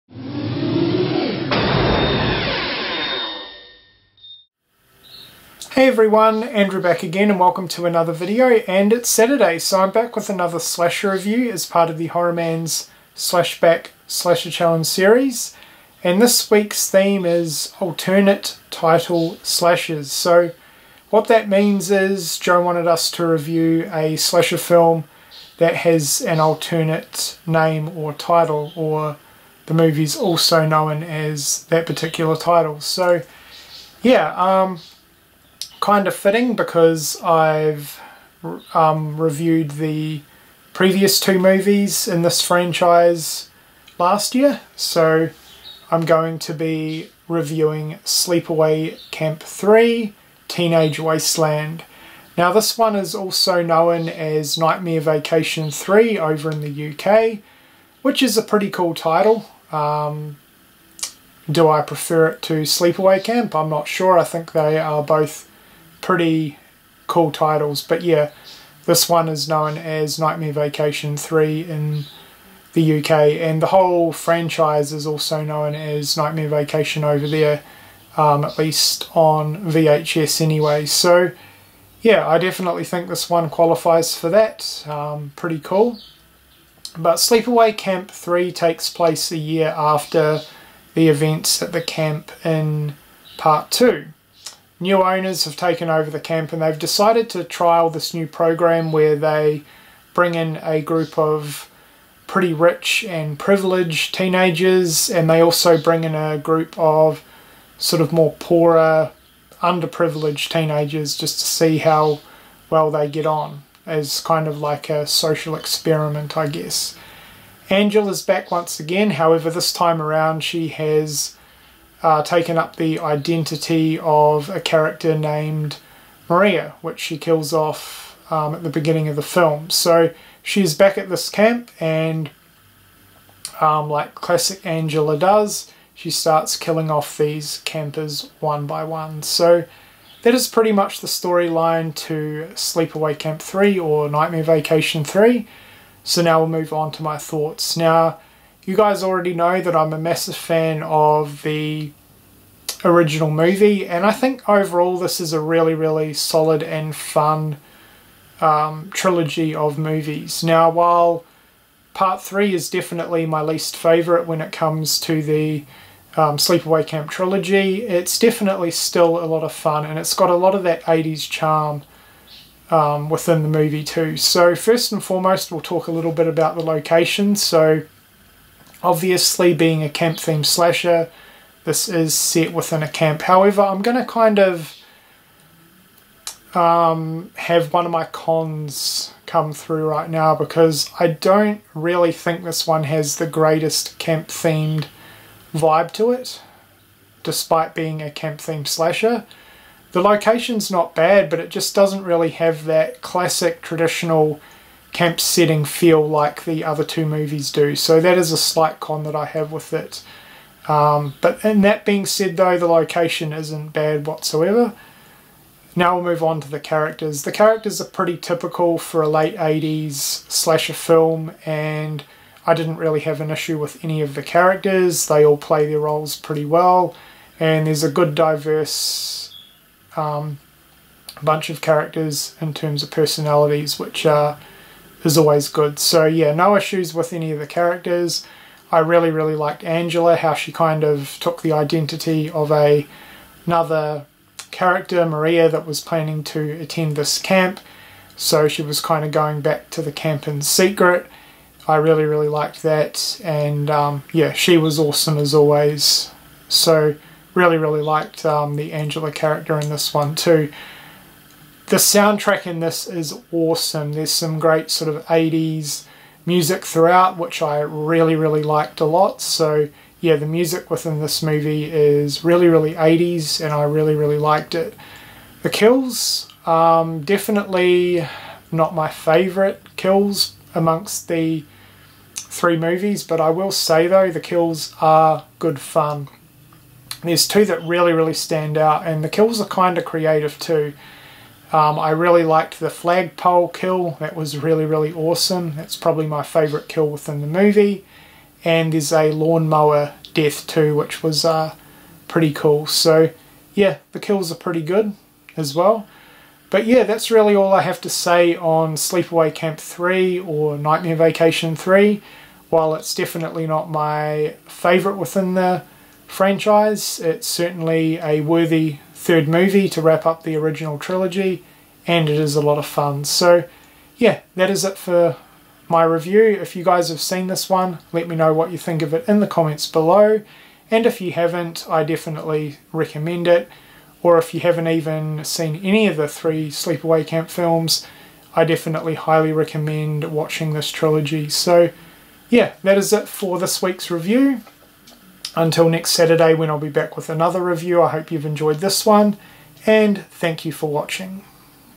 Sleepaway Camp 3 Review - (TheHORRORmans Slashback Challenge)
Here with Week 11 of the Slashback Challenge: The Sequel and this week's theme is "Alternate Title Slashers".  I'm reviewing, Sleepaway Camp 3 - aka Nightmare Vacation 3 (1989).